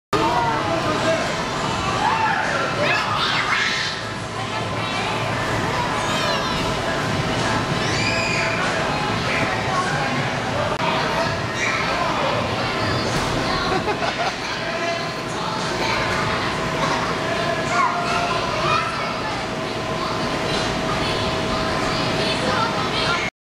Having fun at Pump It Up
Having fun at Pump It Up in Sunnyvale for Dineo's 4th birthday party.